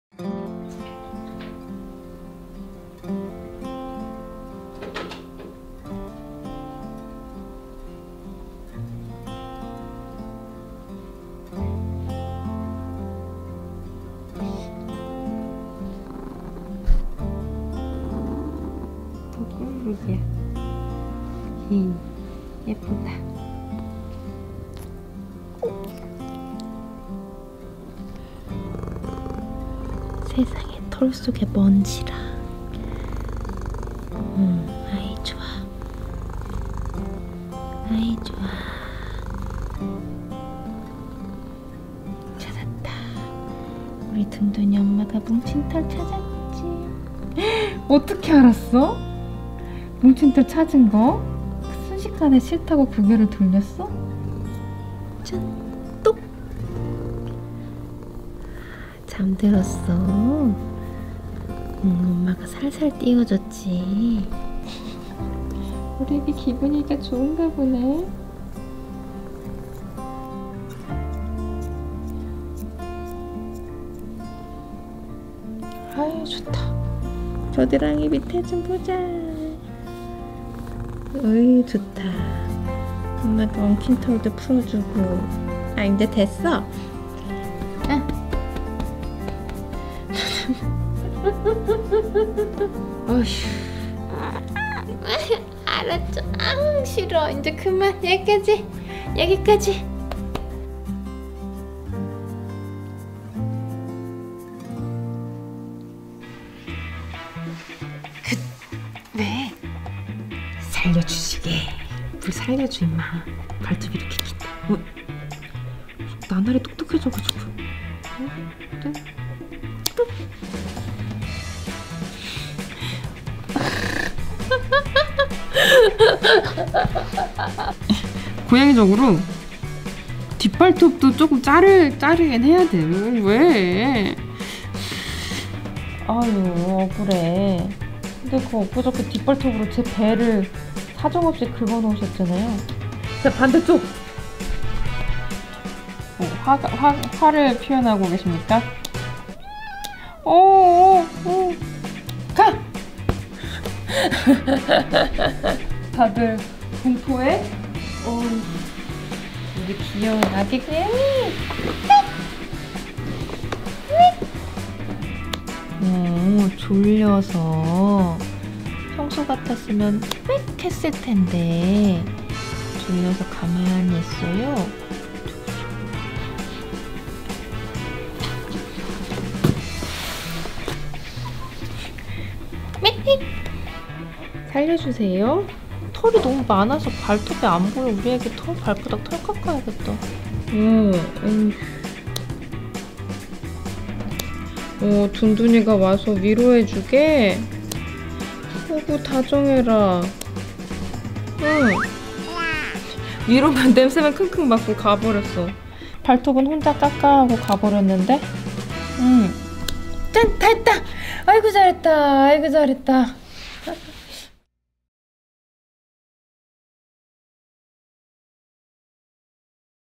꿀잠 자는 고양이 몰래 털 밀어버리기
오늘은 고양이들의 발톱과 털을 정리해줬어요!